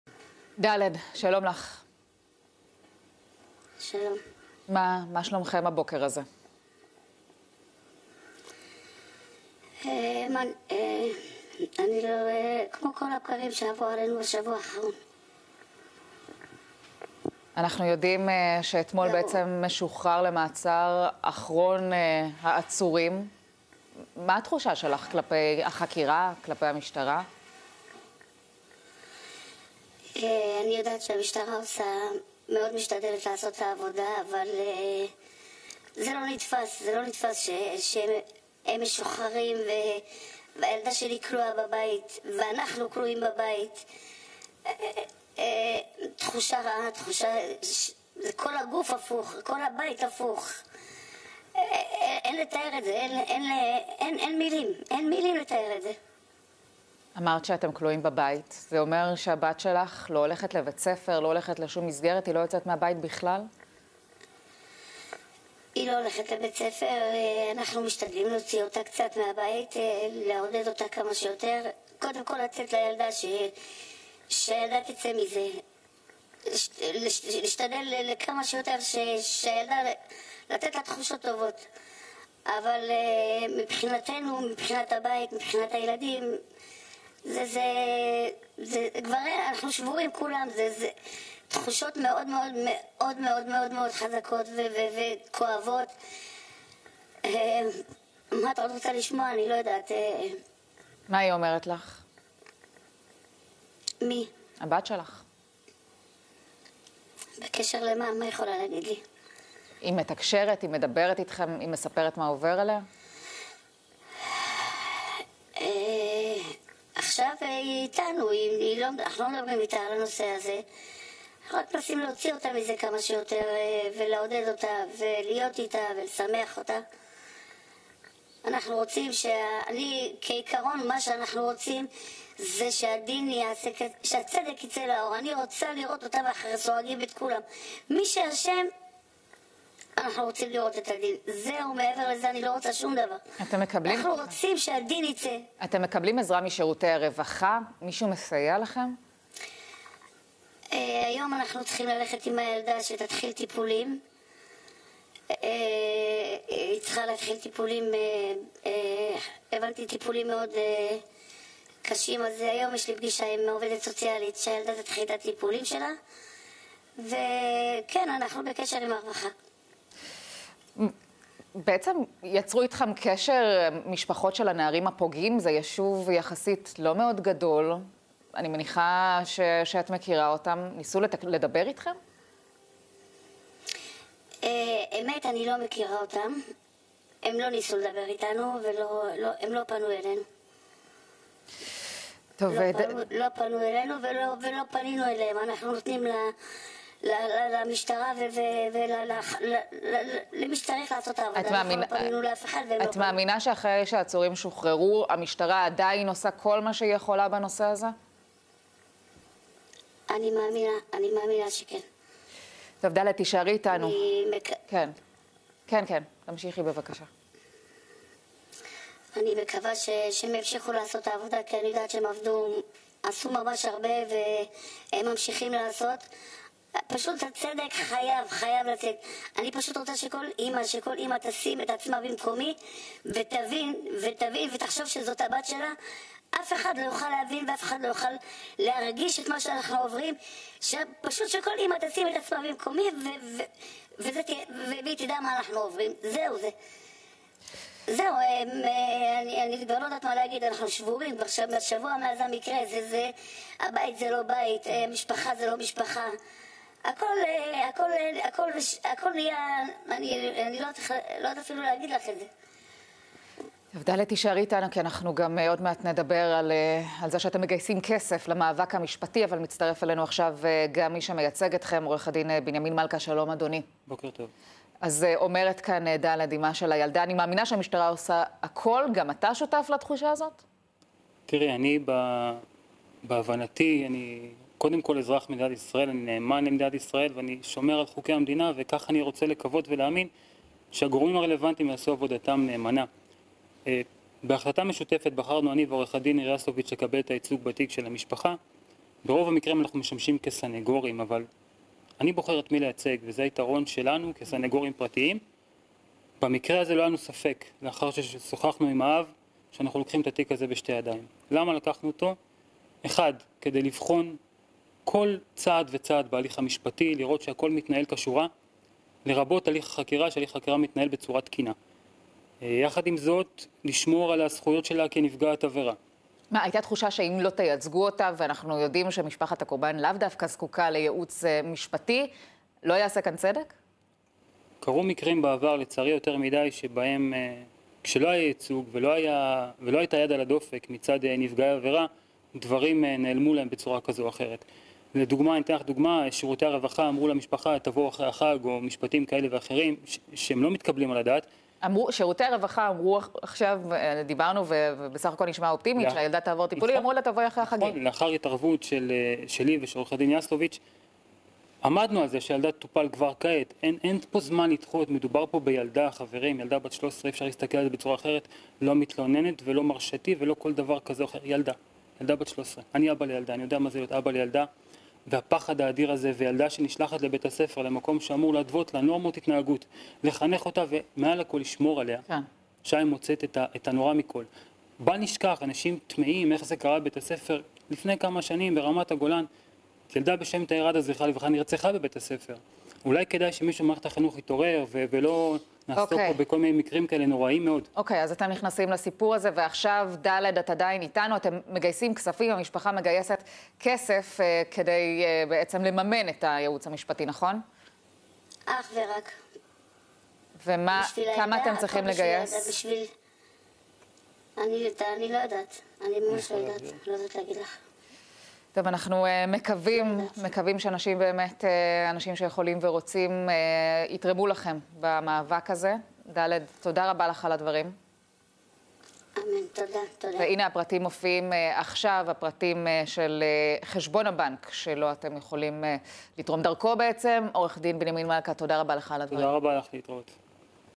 פרשת האונס בדרום: אולפן ynet
ד', אמה של בת ה-13 שלפי החשד נאנסה במקלט בית ספר ביישוב בדרום, סיפרה לאולפן ynet על המשבר שחווה המשפחה: "אנחנו שבורים, זה לא נתפס. הצדק חייב לצאת לאור". עורכי הדין: "החוקר אמר לנערה - תחזרי בך ולא יקרה לך כלום". המשטרה: "האשמות מופרכות"